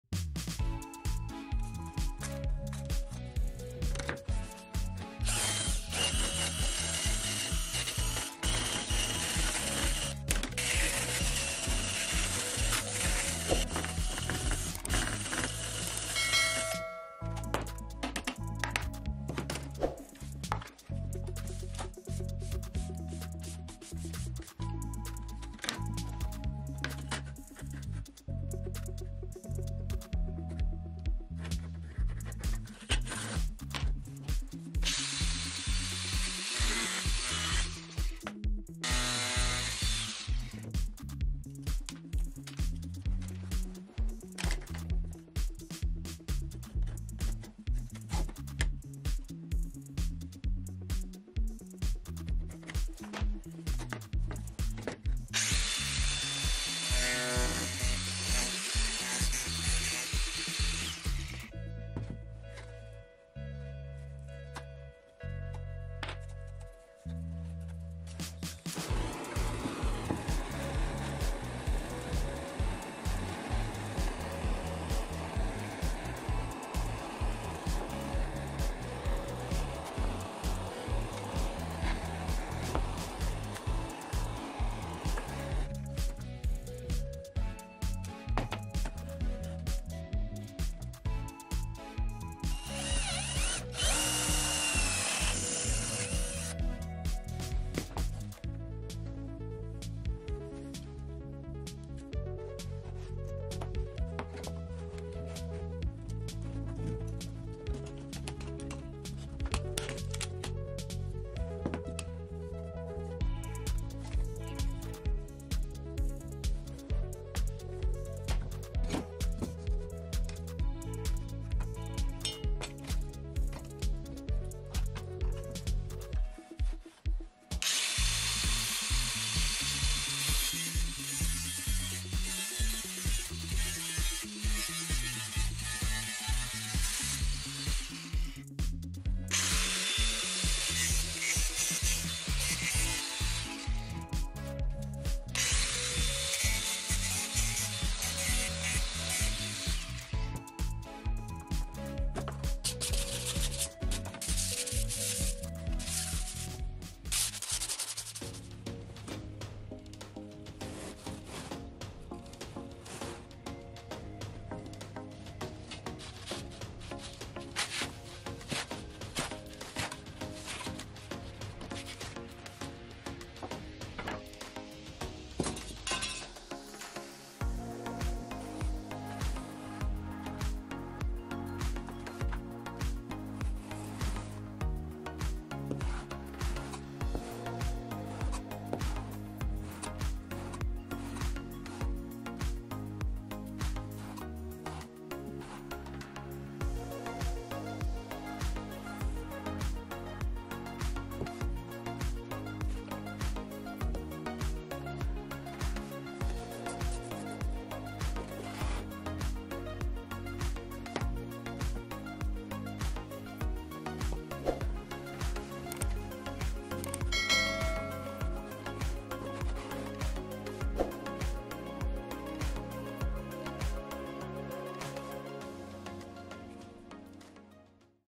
И почему я раньше не сделал этот инструмент, он должен быть у каждого мастера! Самоделка из шпателя!
Телеграм И почему я раньше не сделал этот инструмент, он должен быть у каждого мастера! Самоделка из шпателя! Полезная самоделка своим руками для гаража или мастерской.